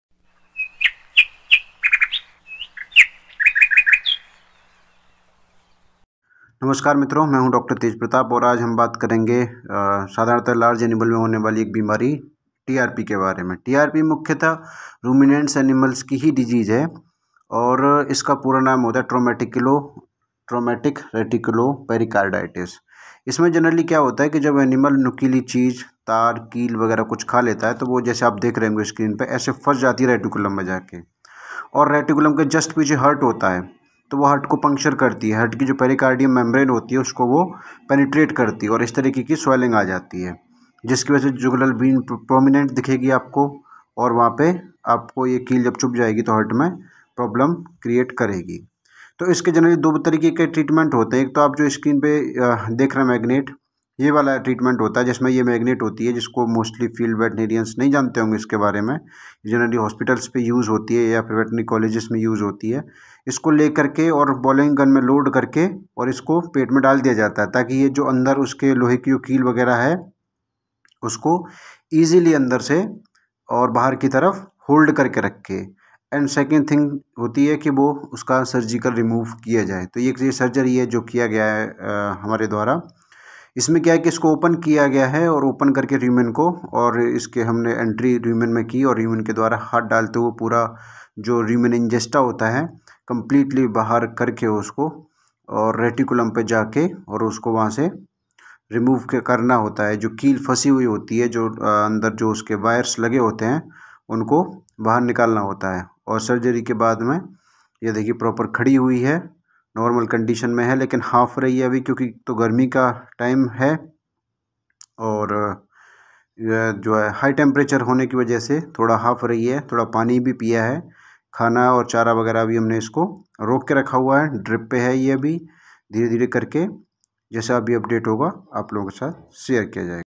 TRP(Traumatic Reticulopericarditis)!!जब गाय या भैंस लोहे की कील,तार् खाने पर क्या होता है?TRP
TRP कैसी  बीमारी होती है सर्जरी और बिना सर्जरी के कैसे होता है इलाज जाने पूरी कहानी,समझे और इलाज का यह तरीका follow करके इलाज कर सकते है